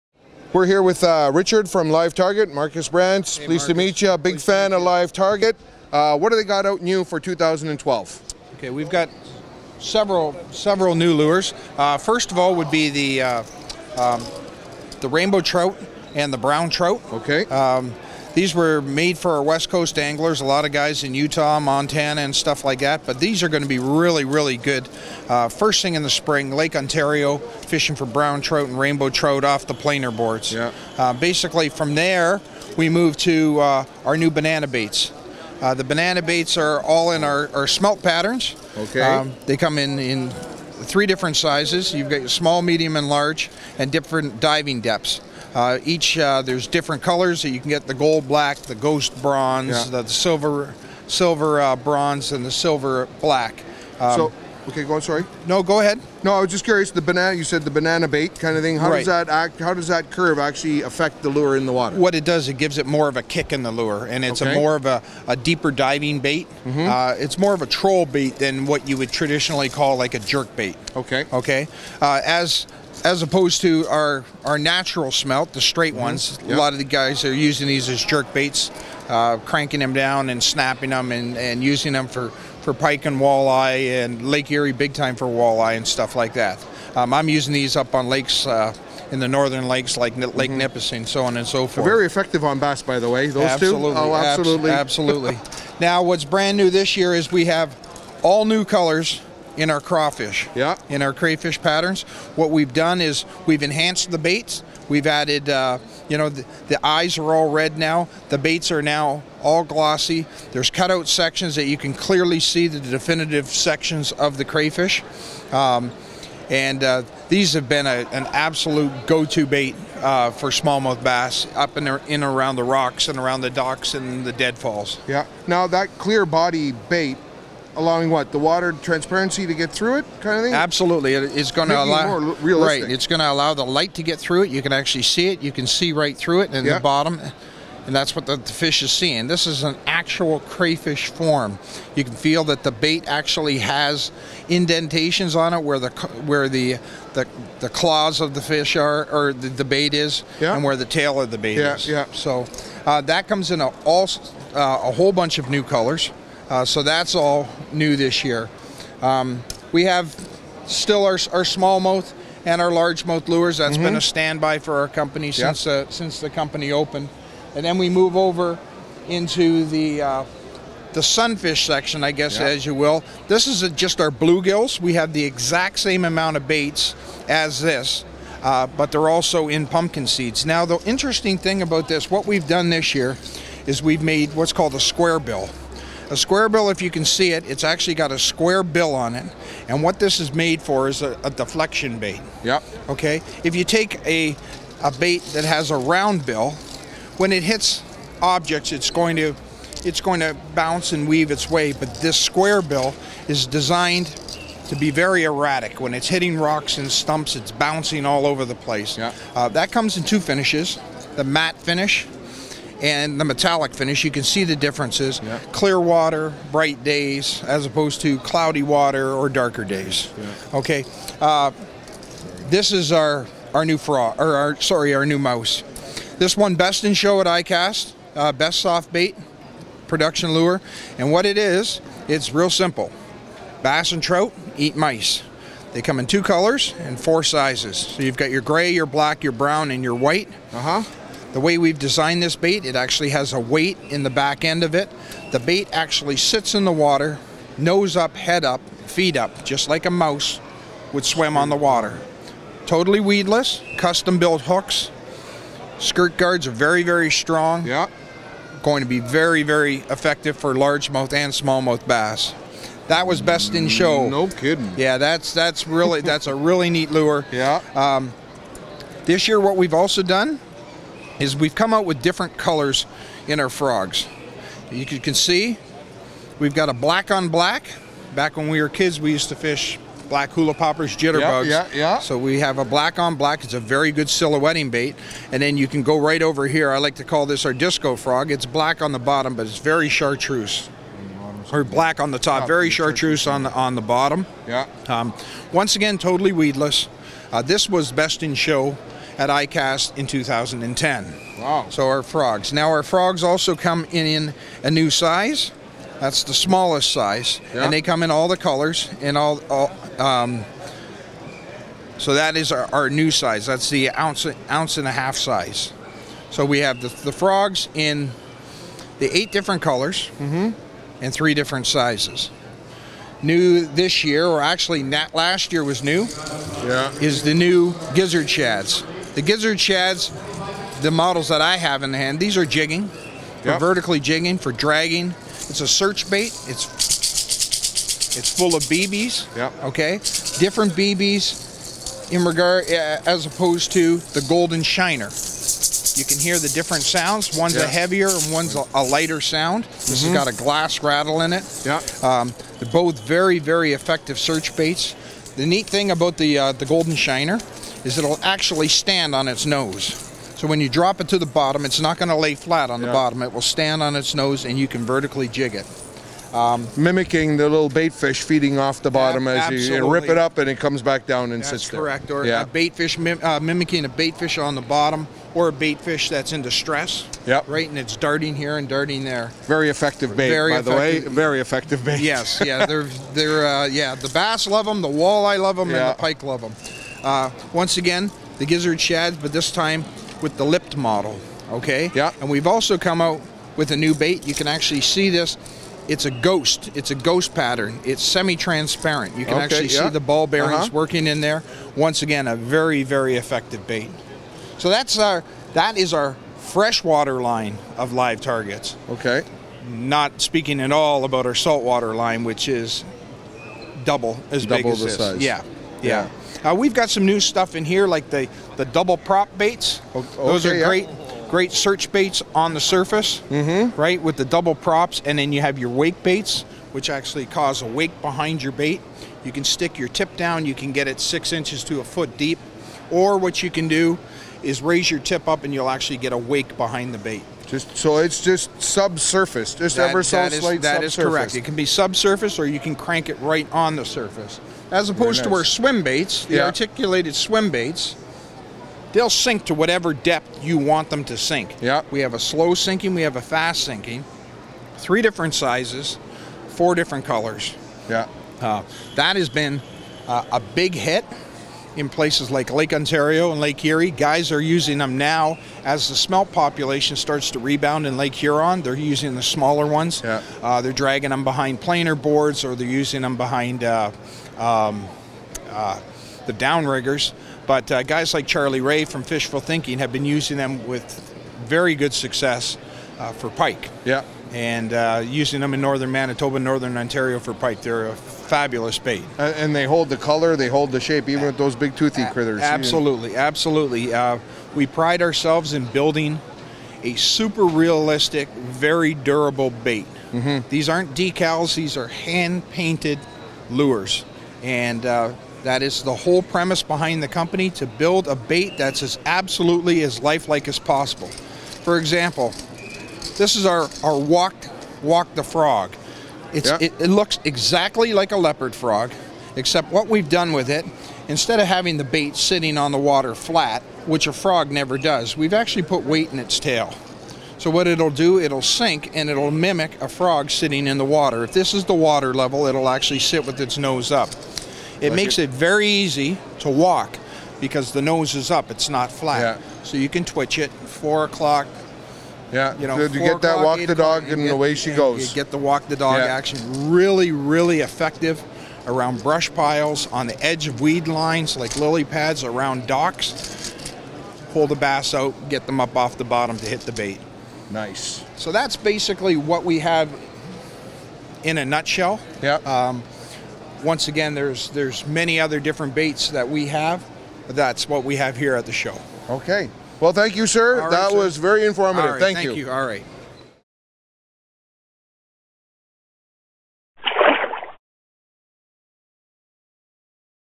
Live Target Life-Like Lures
Richard from LIVETARGET Lures gives Markus Brants of Team BaitMe the scoop on their 2012 Line at the Toronto Sportsmen's Show.